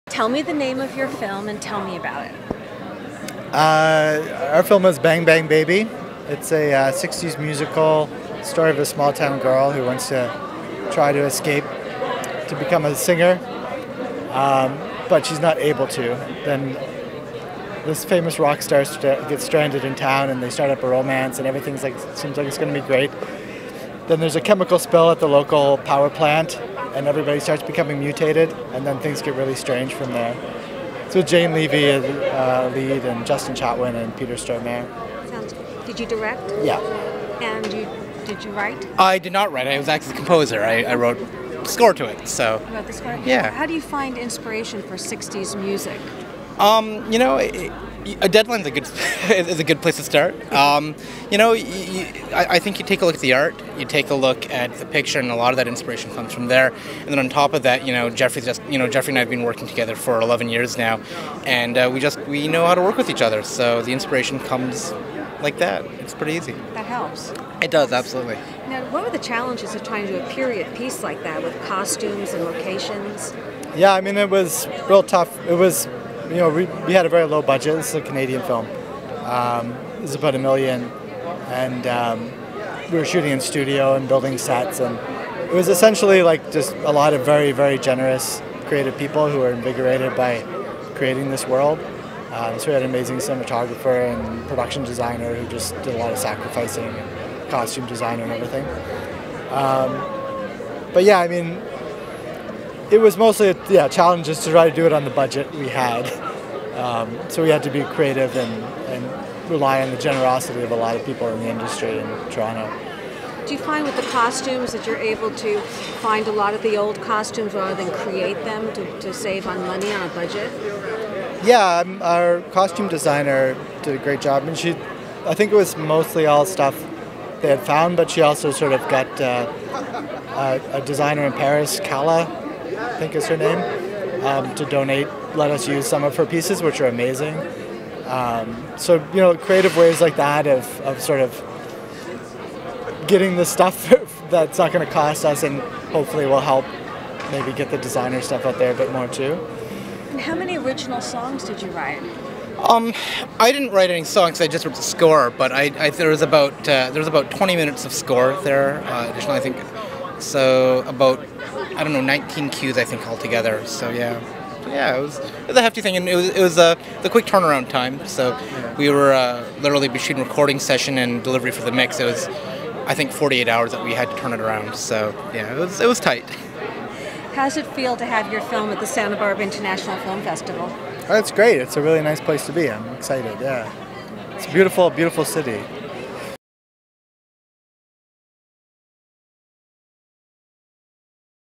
Interview 'Bang Bang Baby' Feature Film at SBIFF
Filmmakers including writer/director Jeffrey St. Jules (r) discuss the film at the Santa Barbara International Film Festival.